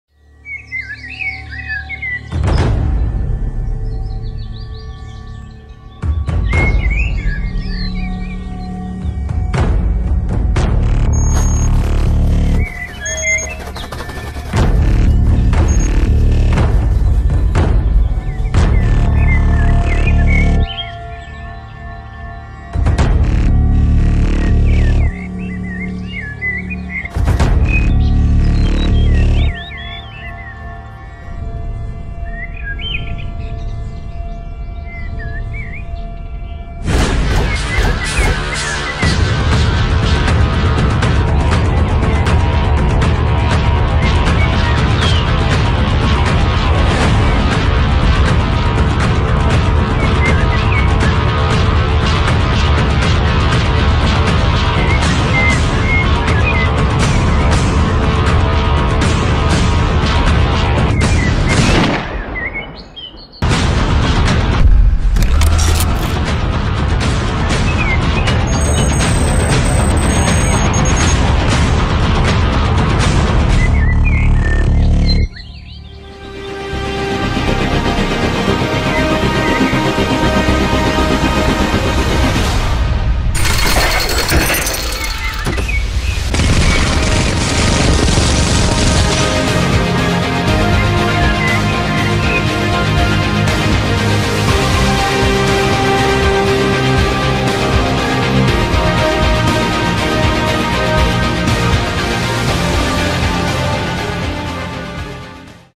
[SFM] Bastion's Last Companion [PC HD]
Starring Bastion & ???

Hey guys, here's another animation short made by me, Alphie Wily.
I hope you all enjoy it as much as i enjoyed making it. :)